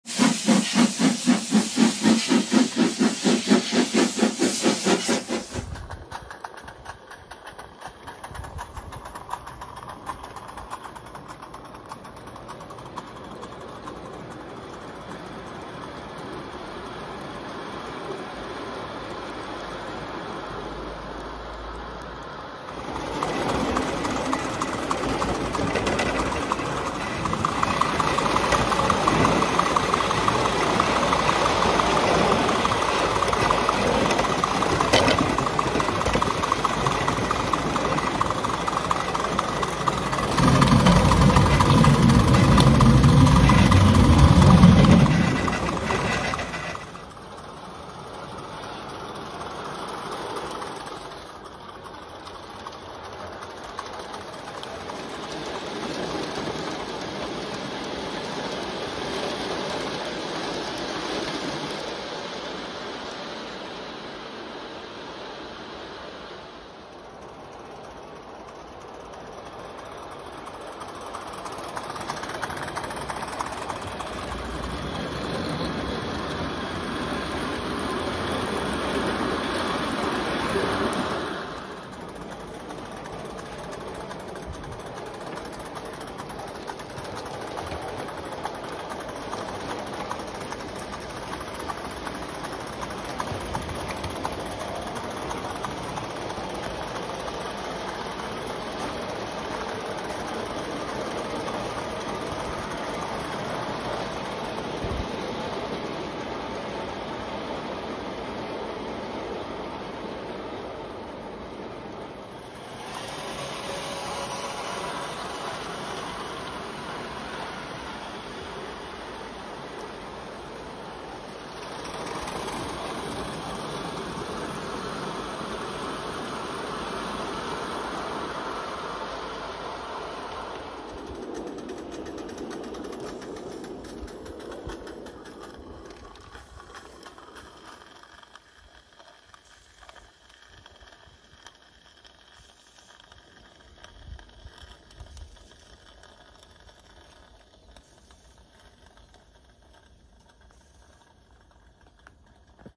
BOWANDE Live Steam locomotive Stanier Jubilee Class. L.M.S. period. Costa Brava, Girona. Spain
Low speed and behind the cabin views. The locomotive is carrying 9 coaches belonging to the live steam lover JL Casacuberta.